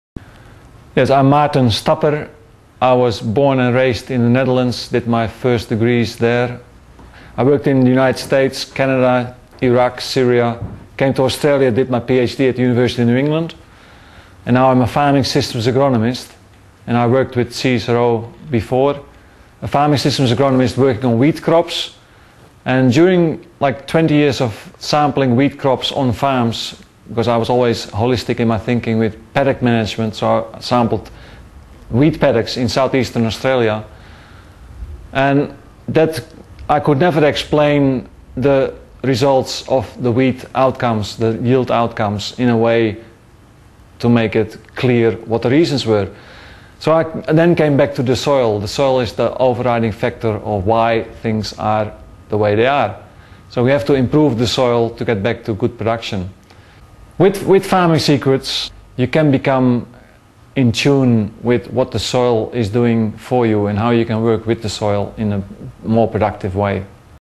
Who is Dr. Maarten Stapper
Maarten is passionate about discovering and using the power of nature in food production systems -- and the connections between soil biology, soil health, and the overall functioning of agro-ecosystems, and sees many opportunities for Australian agriculture to reverse soil degradation and regenerate soils.